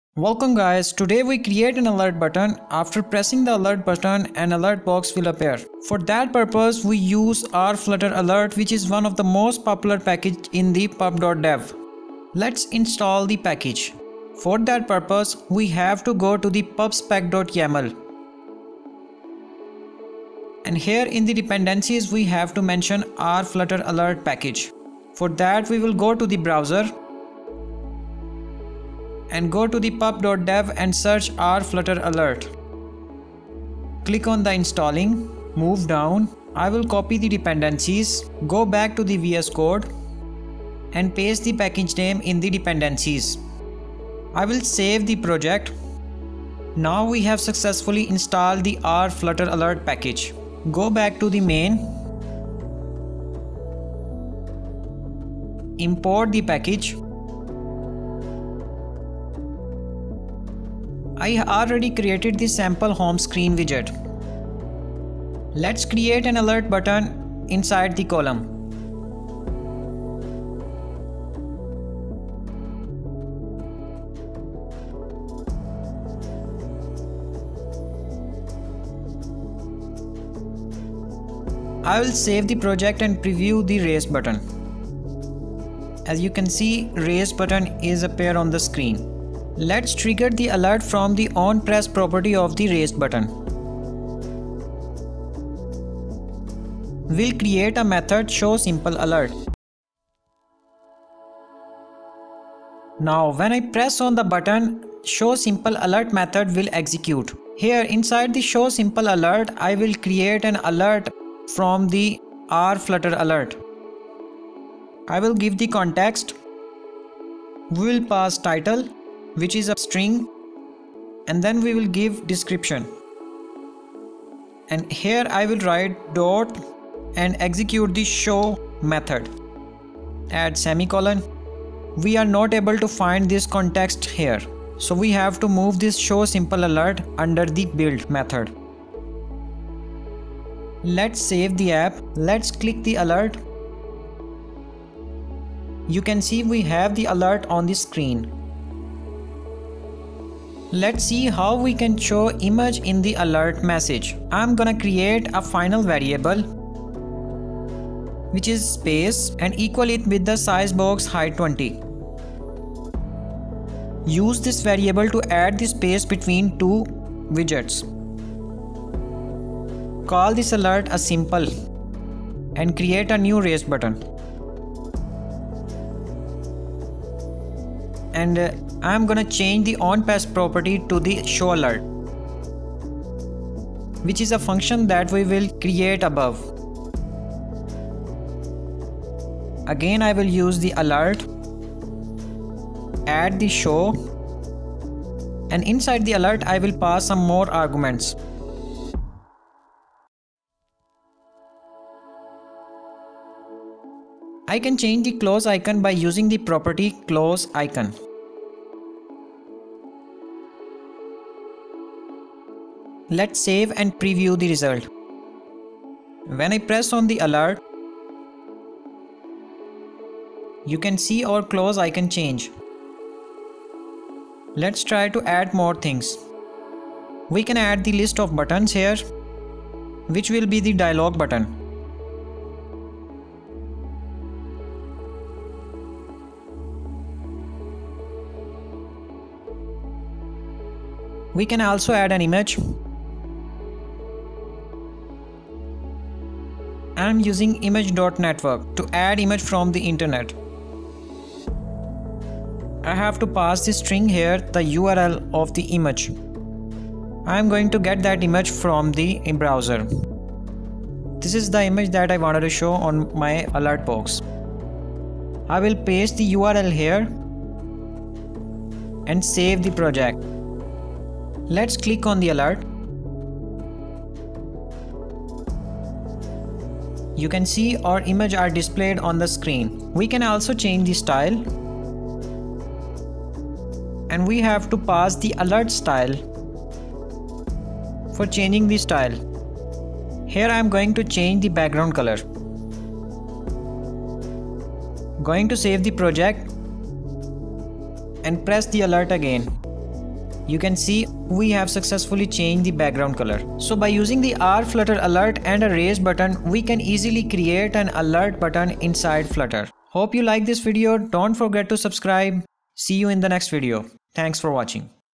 How to create an Alert Button in Flutter - RFlutter Alert | Flutter Alertdialog
In this video we will learn how to create an alert button in flutter, so that when a user click on that button an alert dialog box will appear. We use rflutter_alert package.
There will be 2 different type of flutter alert dialog examples in which an alert message is shown with beautiful fade animation.
RFlutter Alert package give us the alert widget and the widget is totally customizable. These customization will also discussed in this video.
flutter alert dialog with button
If you have any question. Please write in the comments!